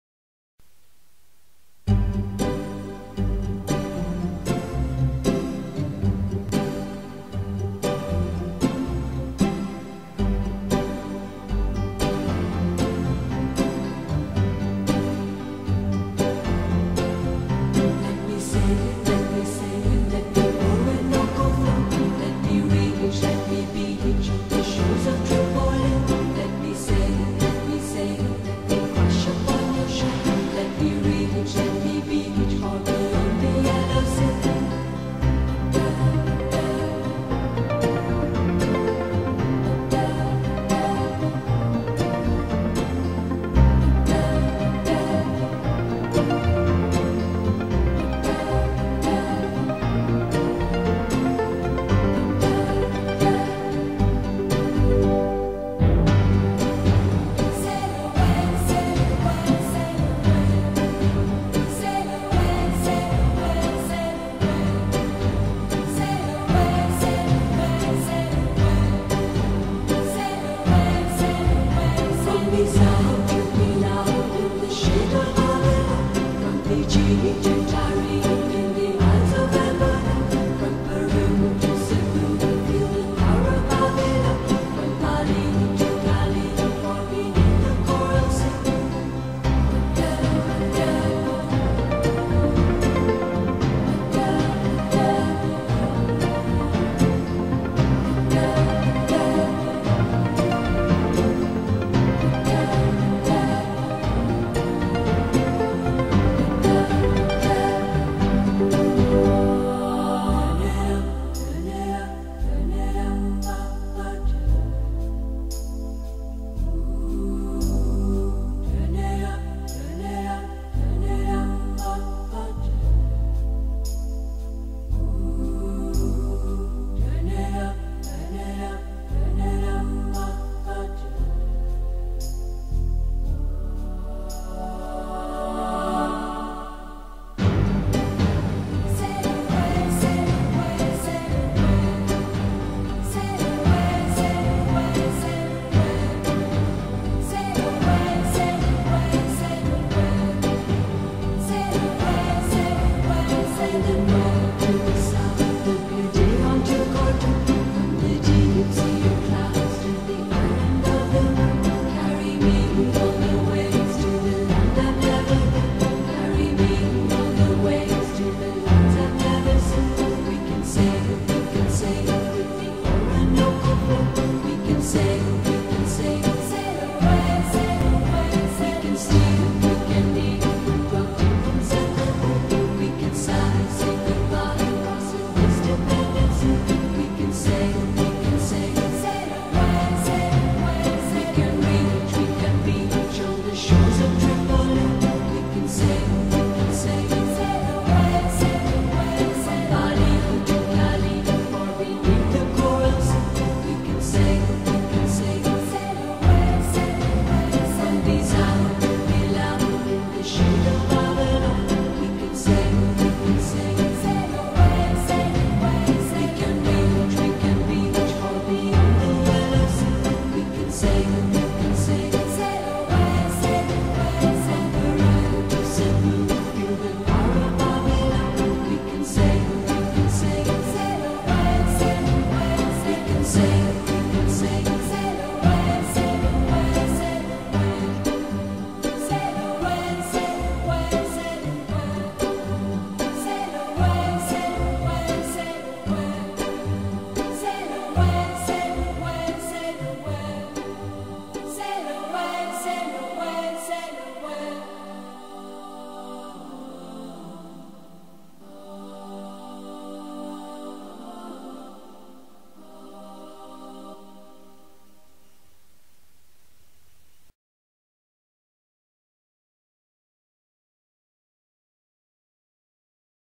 aida bella malta
aida bella in haven van Valetta op 31 december 2009
blusboot heet ons welkom